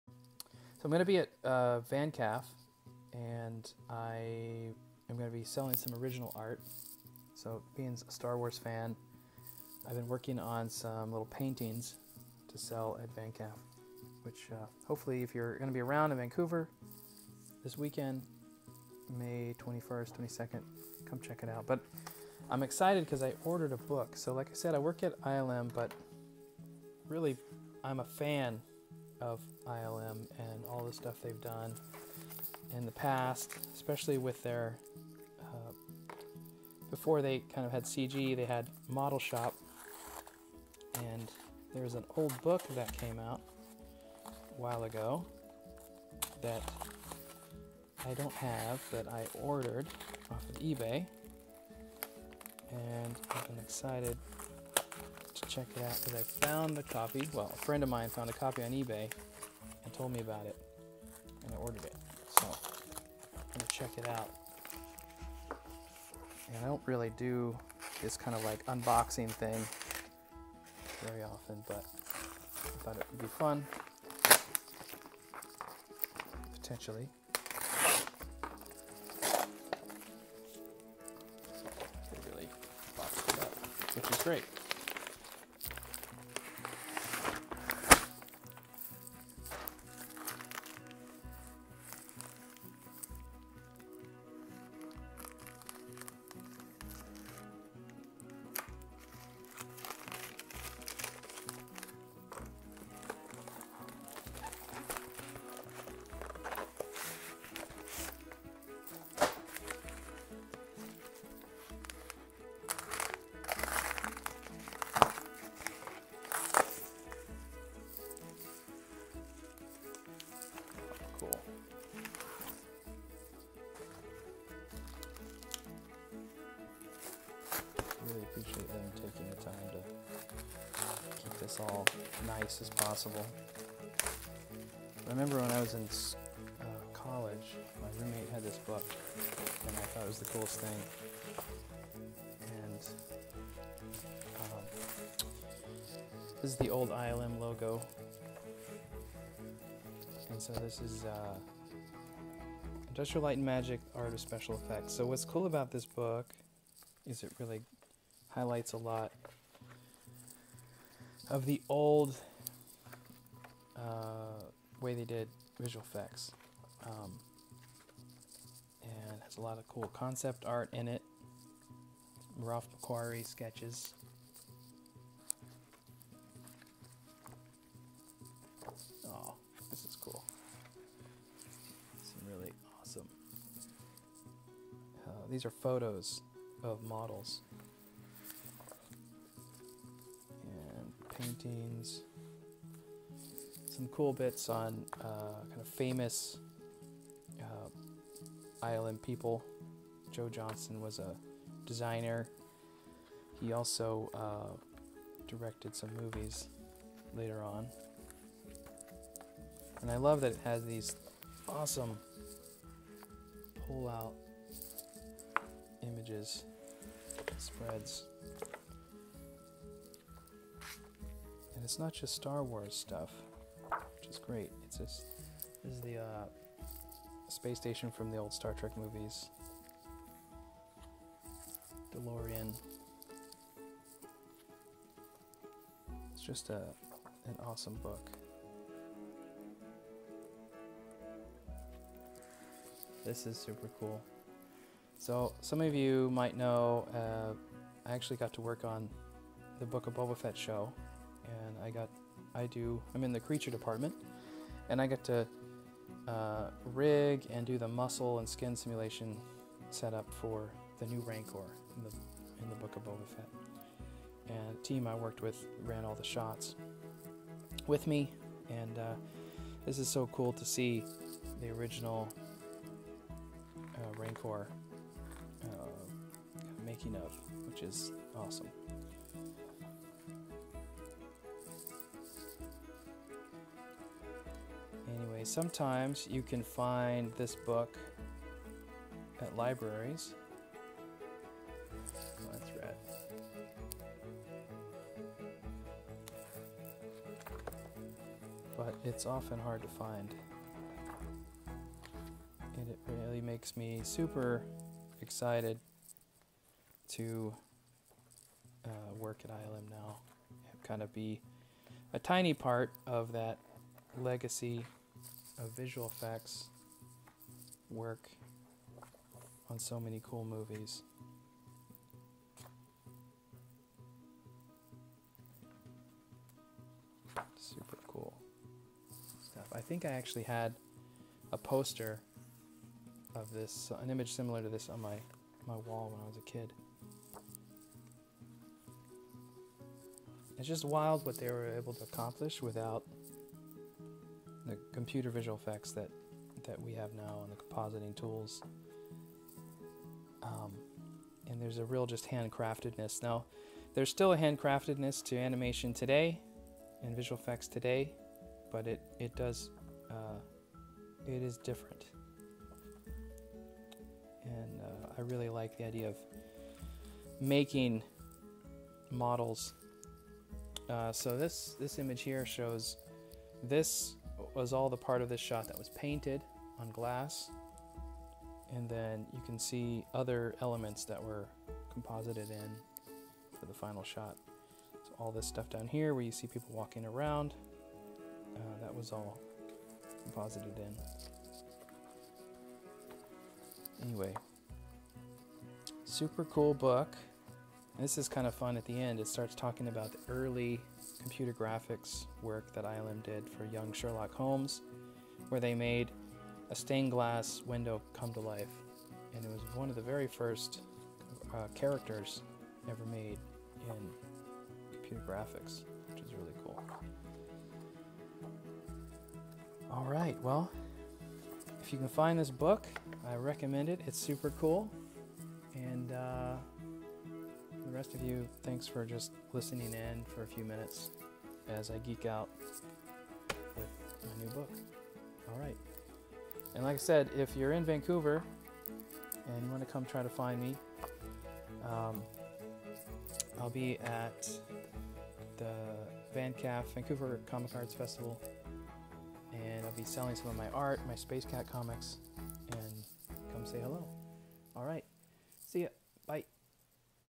Unboxing some VFX history - Industrial Light and Magic - The Art of Visual Effects book walkthrough
Let’s walk through this rare and amazing book about the early years of Industrial Light and Magic